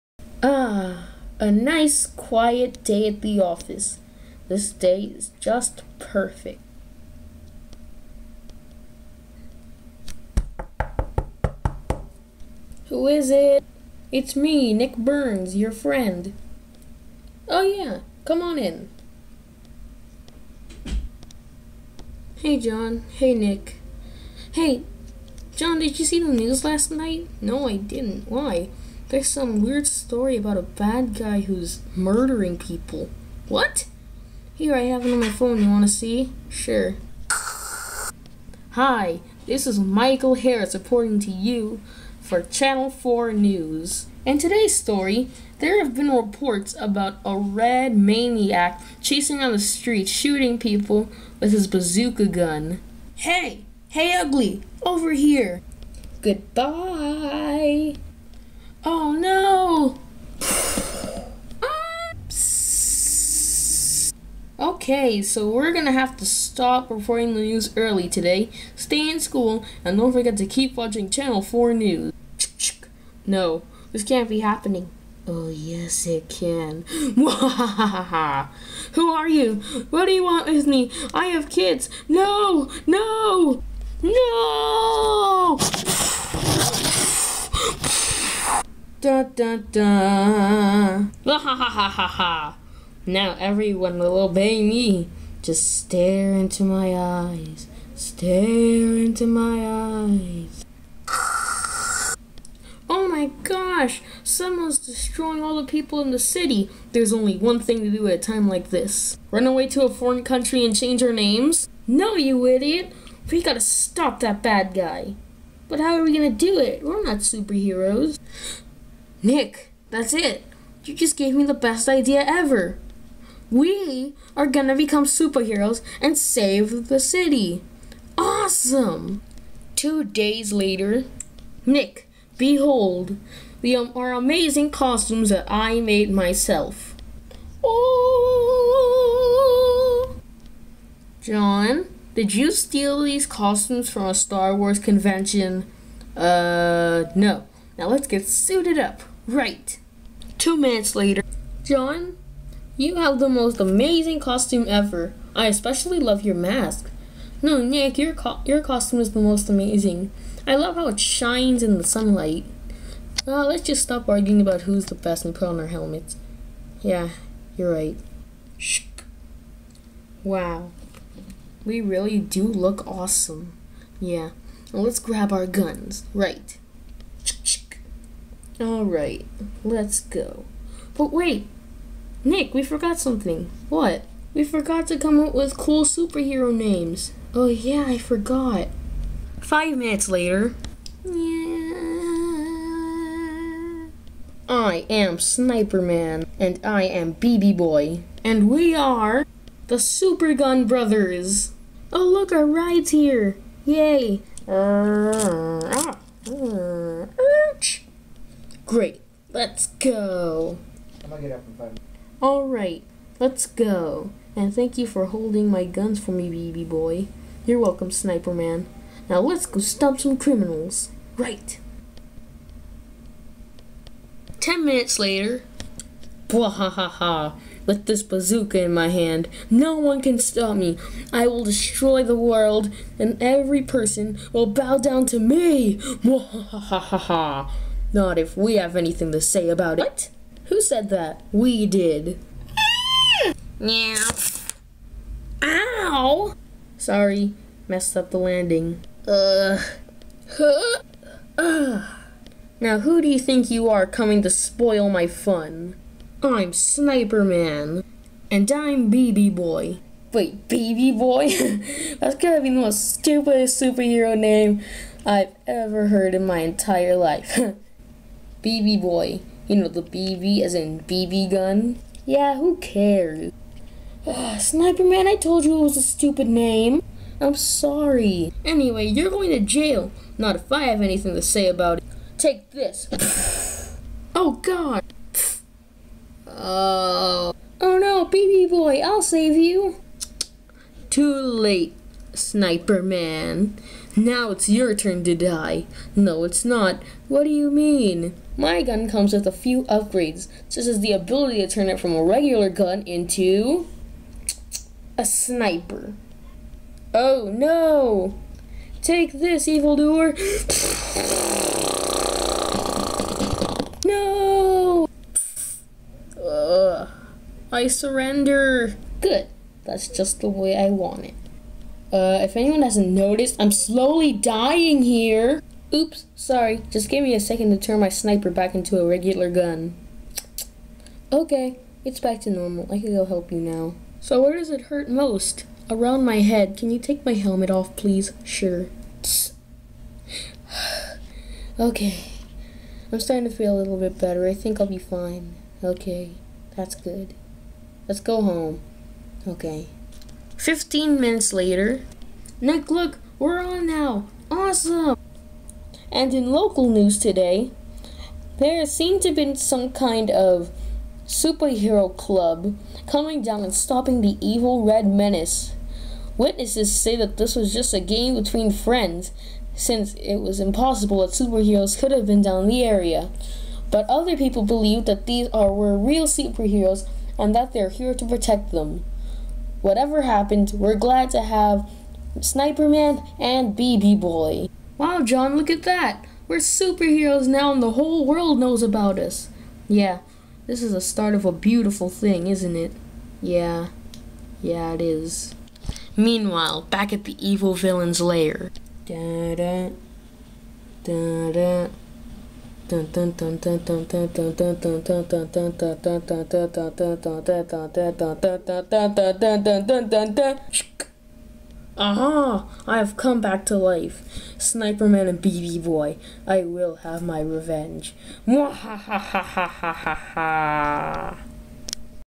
The Adventures of Sniperman and B.B.Boy! Episode 1: Birth of a superhero
In this series opener, policeman John Michaelson and his best friend Nick Burnes become superheroes to stop a madman killing everyone in the city.